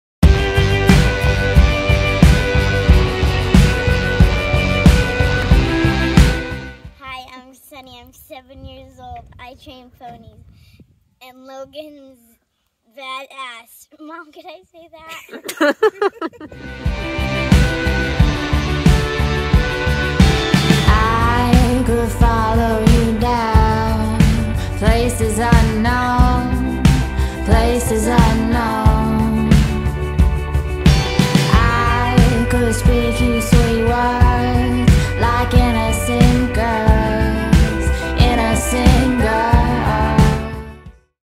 Ep. 1 - Sunni - Friendly Tomcat Has Lots of Friends (Diamonds in the Desert Lot 55)
Sunni, 7 year old pony trainer, takes Logan to town and then gets him loaded up to head home. Hard to say which one of them is cuter!

Logan is a four year old gelding by Tomcat Chex. He's consigned to the Diamonds in the Desert Premier Horse Sale in Reno, NV. More info at syringabendstockhorses.com or sale info at premierhorsesales.com